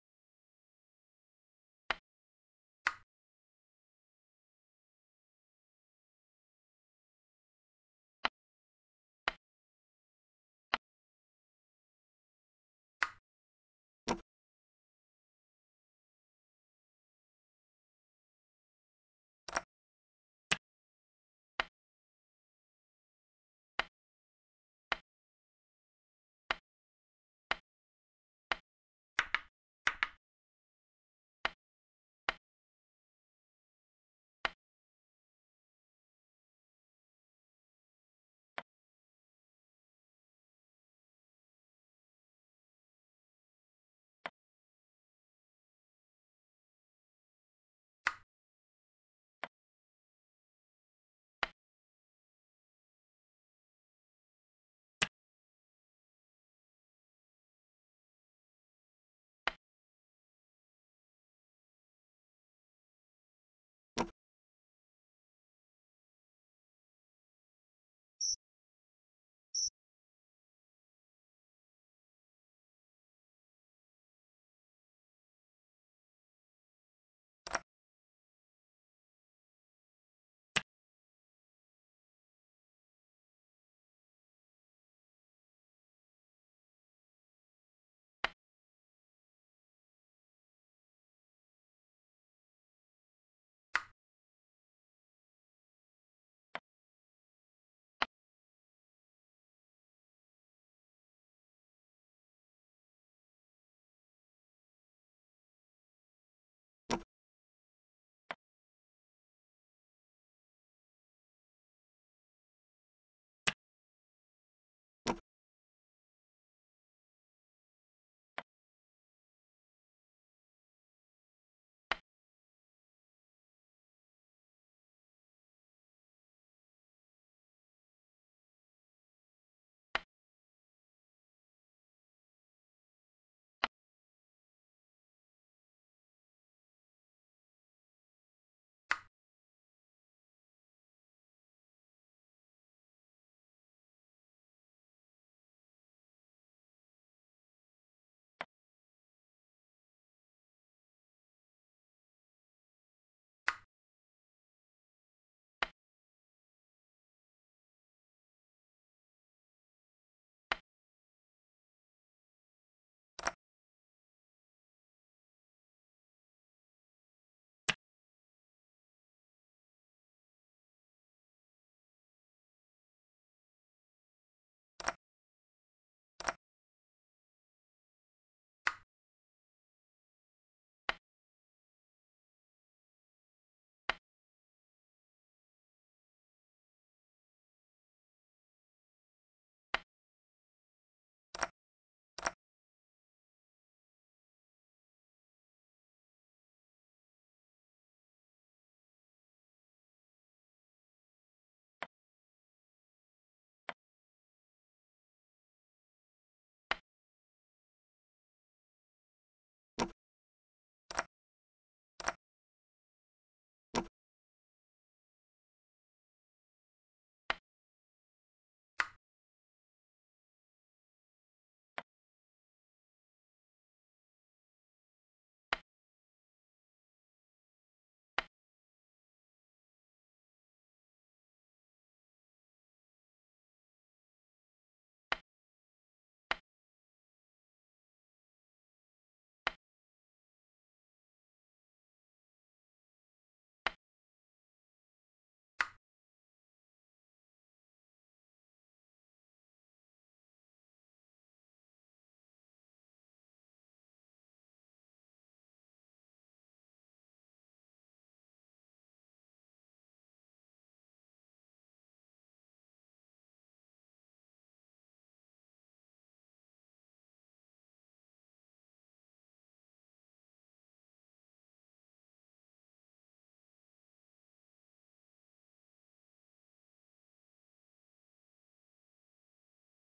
Series 1 || Tarrasch Defense || 9.Bg5 with 10.b3
The Tarrasch Defense is a chess opening characterized by the moves: 1. d4 d5 2. c4 e6 3. Nc3 c5. The Tarrasch is a variation of the Queen's Gambit Declined.